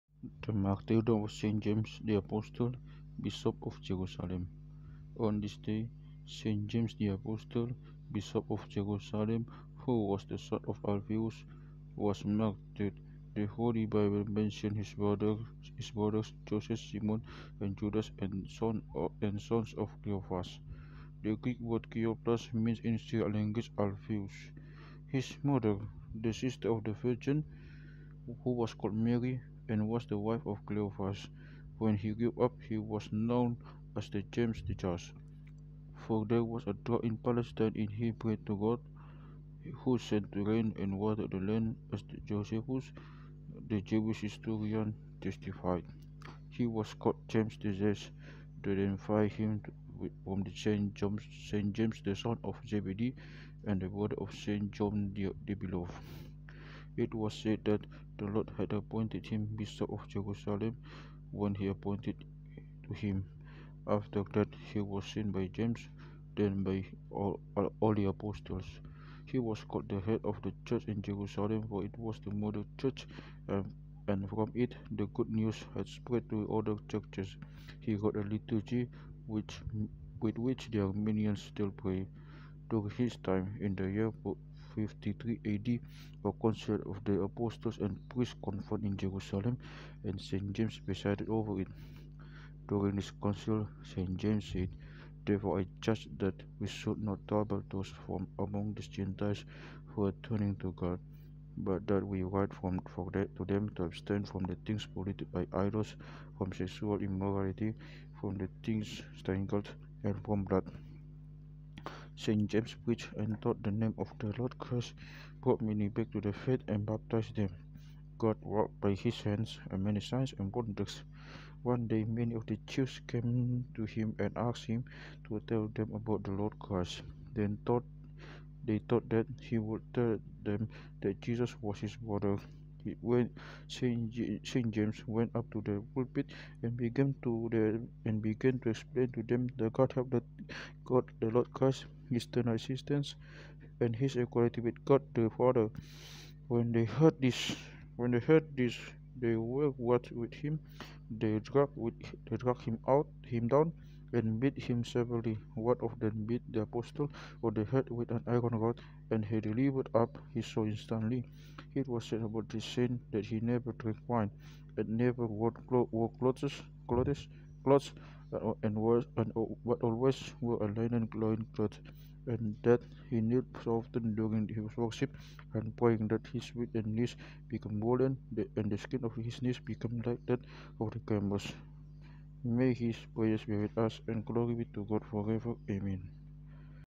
Coptic Synaxarion 18 Epep (July 25th)
1. The Martyrdom of St. James the Apostle, Bishop of Jerusalem

I apologize for wrong pronunciation, because I don't speak English as my native language